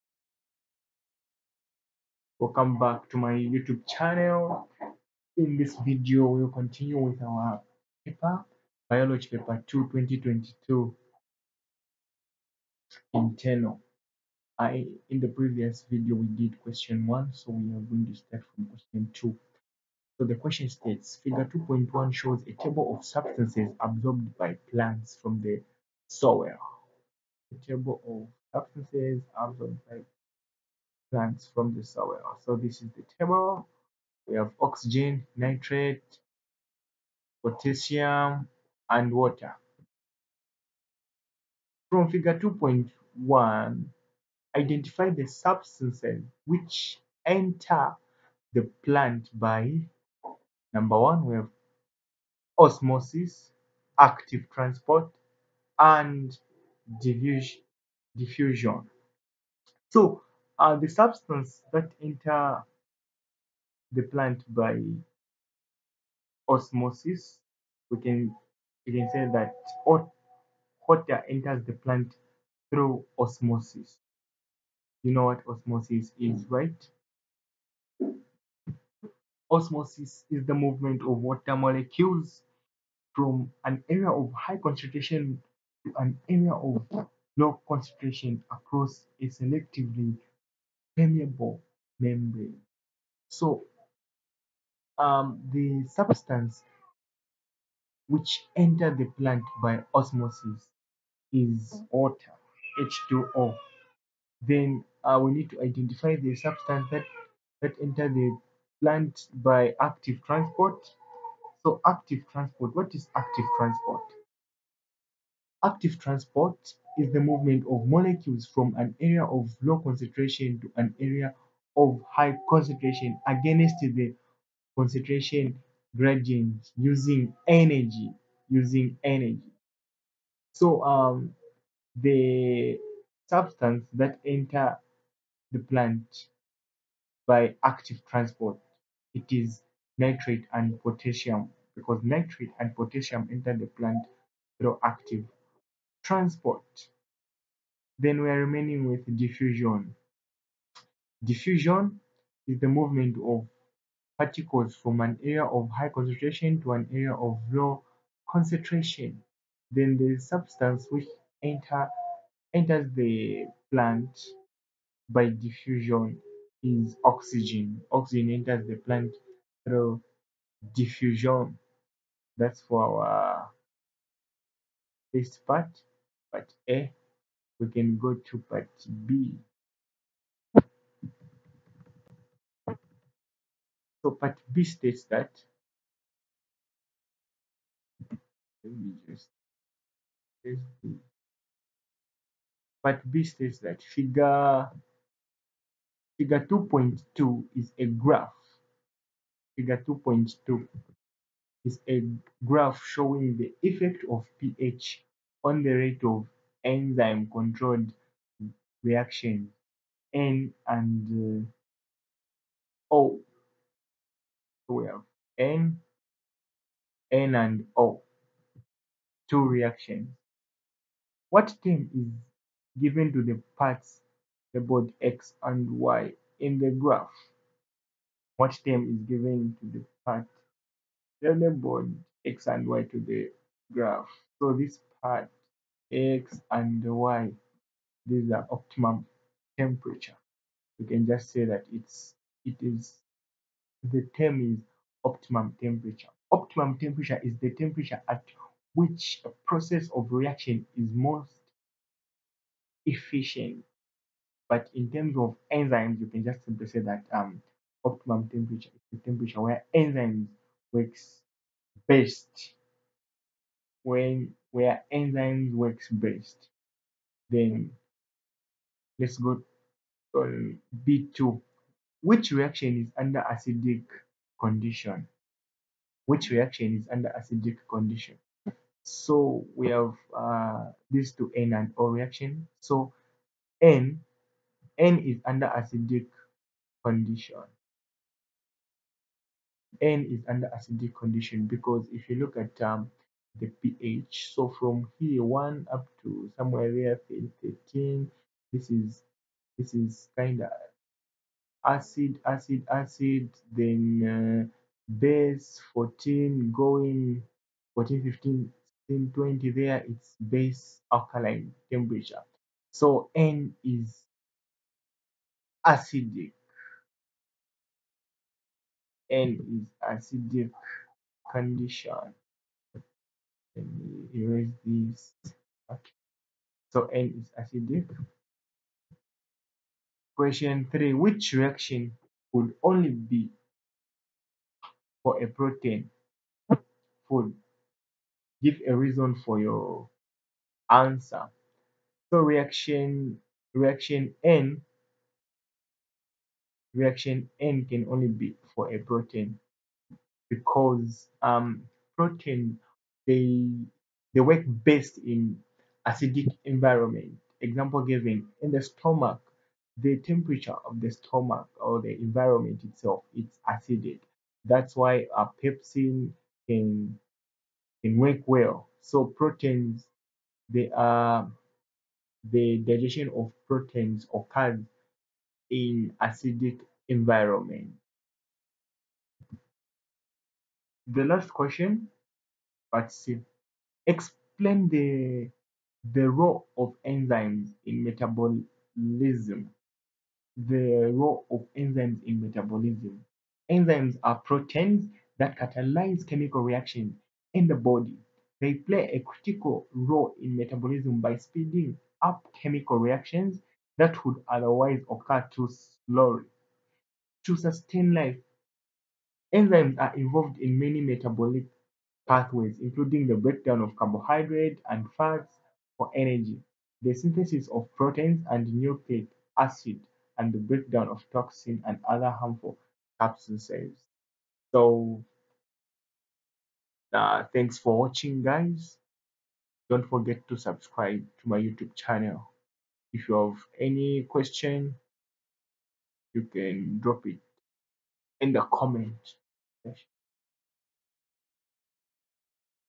eczbiology biology paper 2 2022 question 2
Oxygen enters the plant through diffusion. Nitrate and potassium enter the plant through active transport. Water enters the plant through osmosis.
Enzymes are proteins that catalyze chemical reactions in the body. They play a critical role in metabolism by speeding up chemical reactions that would otherwise occur too slowly to sustain life. Enzymes are involved in many metabolic pathways, including the breakdown of carbohydrates and fats for energy, the synthesis of proteins and nucleic acids, and the breakdown of toxins and other harmful substances.